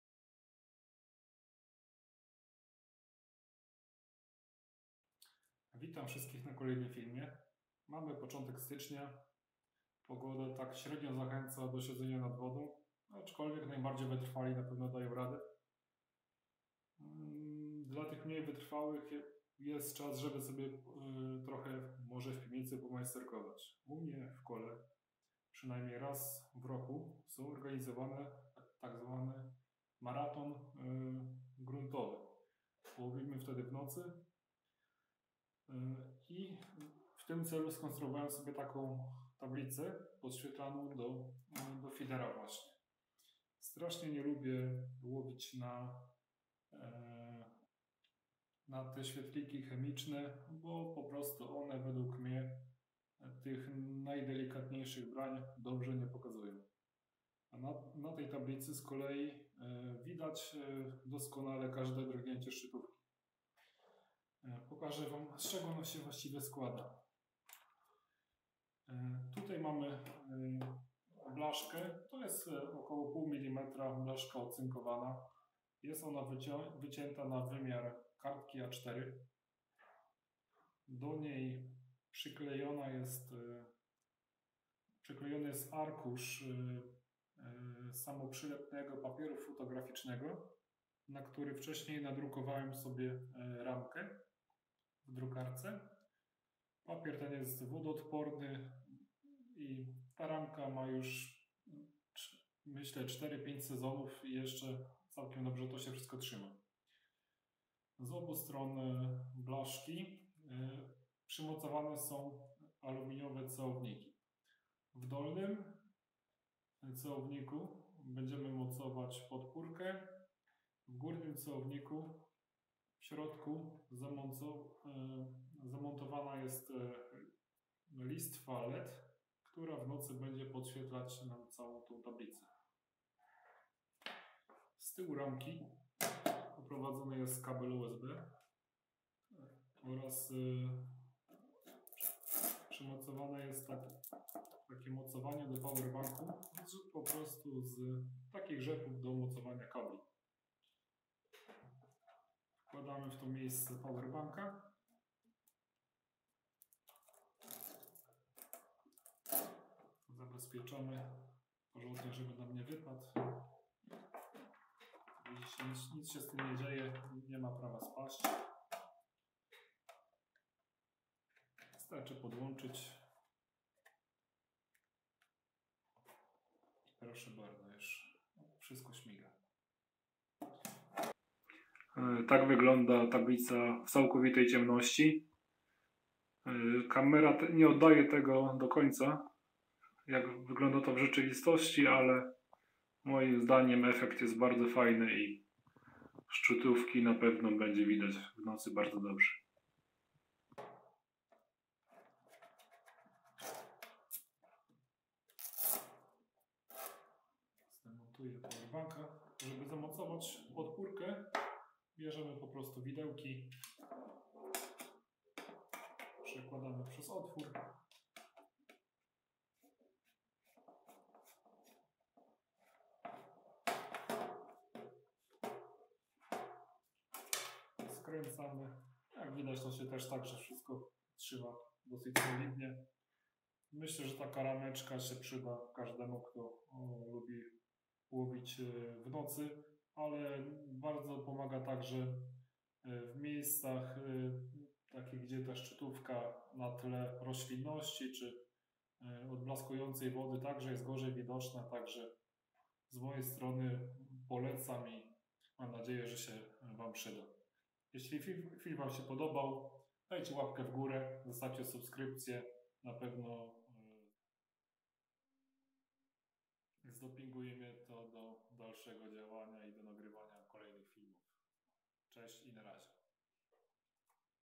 Podświetlana tablica do feedera. Fishing targed board
Krótki poradnik jak bardzo prosto można ułatwić sobie obserwacje szczytówki feedera zarówno w nocy jak i w dzień.
Film się podobał. Daj suba i oglądaj inne moje treści.